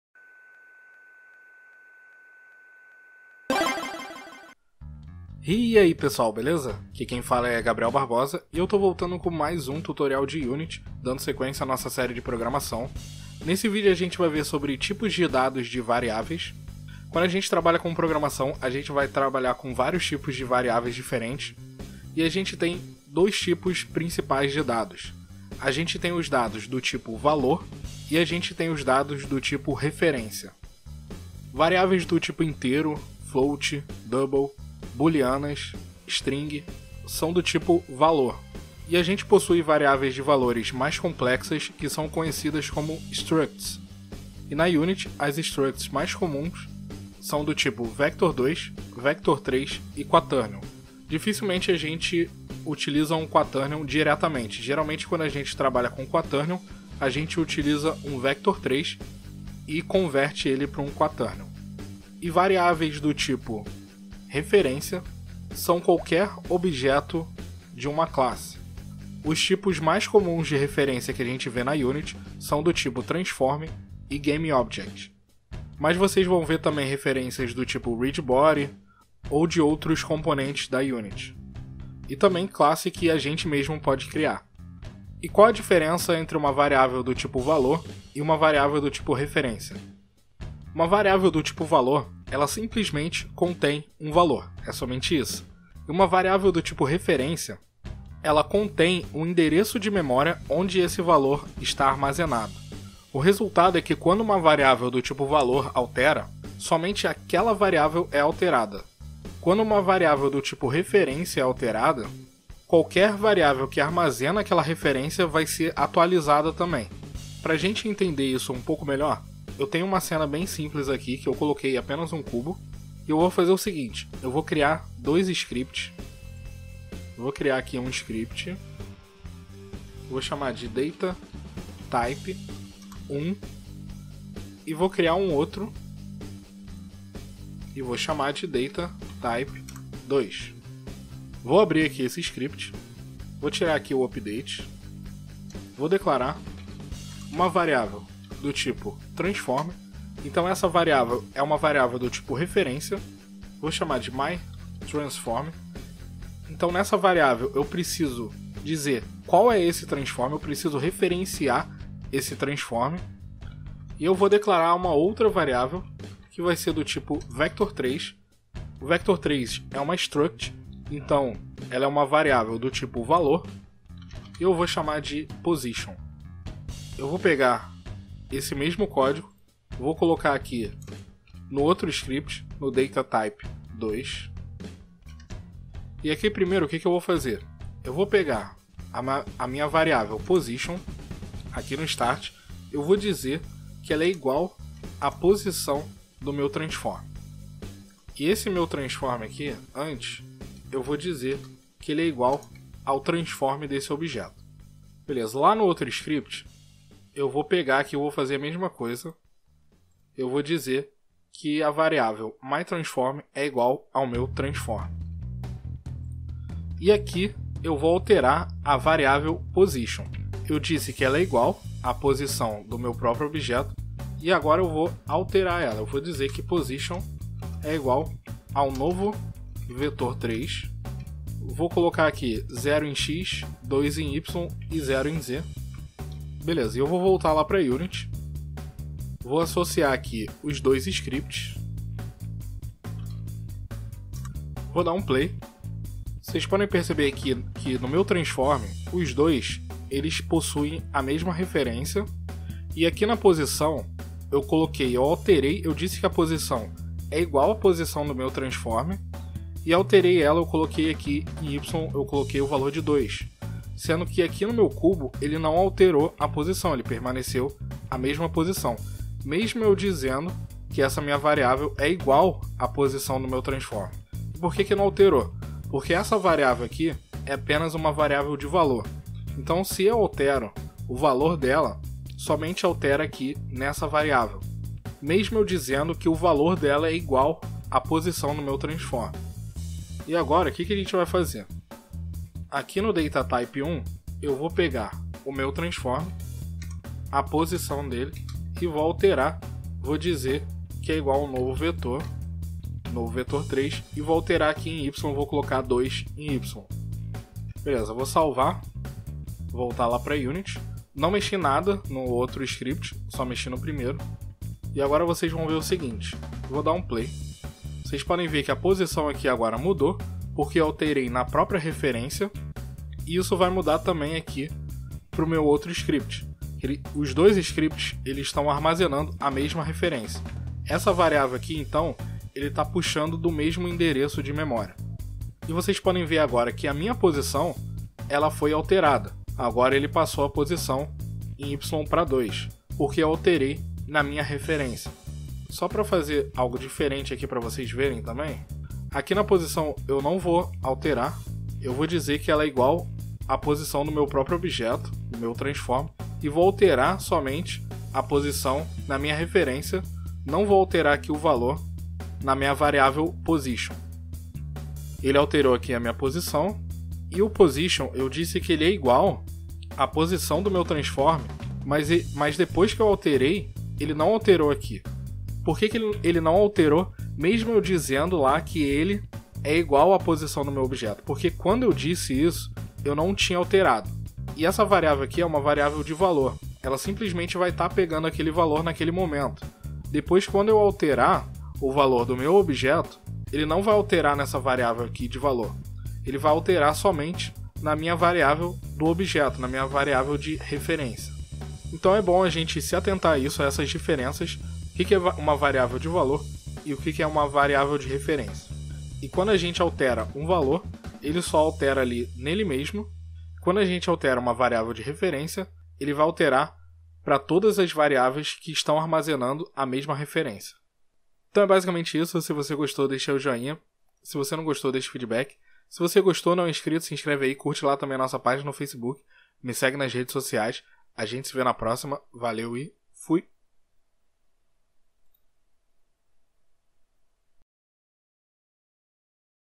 Tipos de dados (Valor e referência) | Programação na Unity
Nesse vídeo vamos ver sobre dois principais tipos de dados de uma variável. Variáveis que são do tipo valor e variáveis do tipo referência.